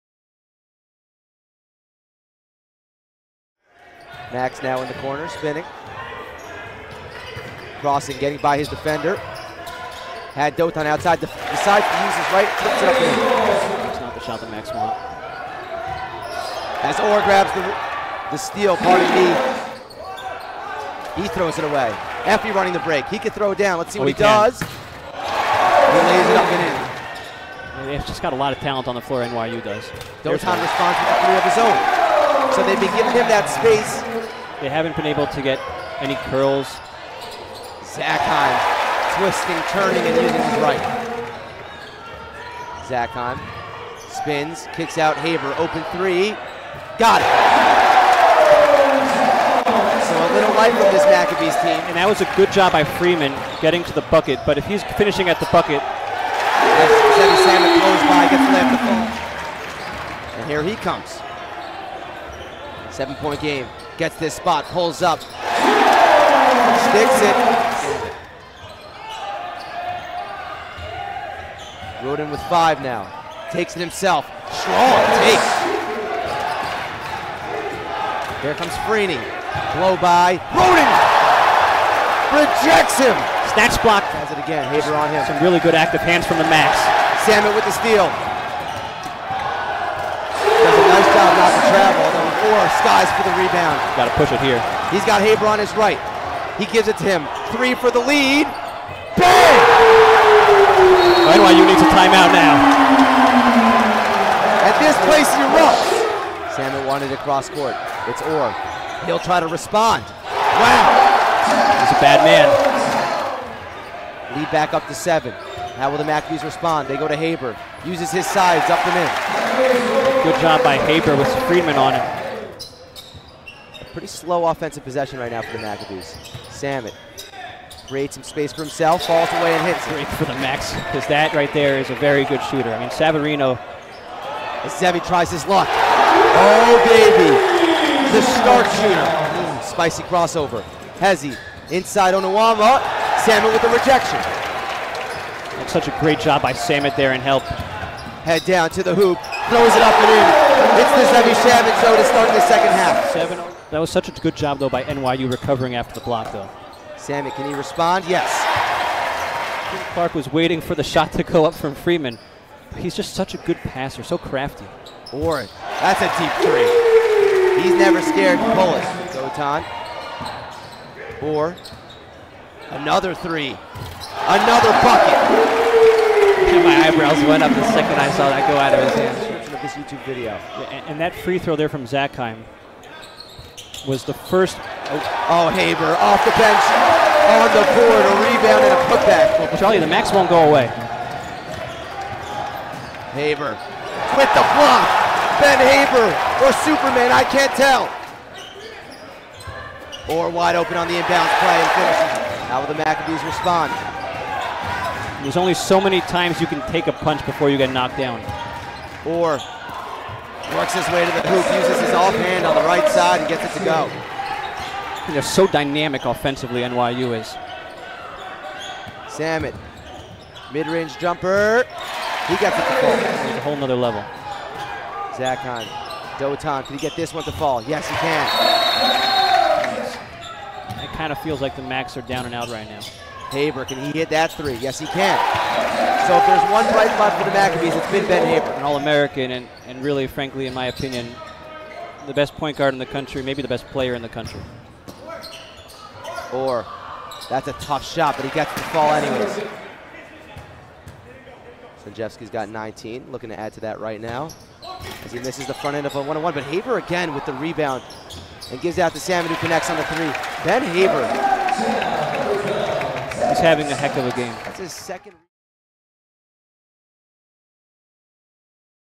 MBB Highlights: YU Macs VS NYU Violets 11/18/2023
NYU Violets Beat YU Macs 97-78

To stay in the know with all things Macs, join our WhatsApp Group Chat:

Executive Director: Eitan Traurig
Executive Producer: Zevi Pancer
Director: Ezra Jacobs

Play-by-Play: Charlie Bendheim
Color Commentator: Akiva Poppers
Sideline Reporter: SJ Tannenbaum

Main/Wide Cameraman:  Jarret Lazarus
Iso Cameraman: Aharon Traurig
Right Floor Cameraman: Gavi Gershov
Left Floor Cameraman: Yosef Bluth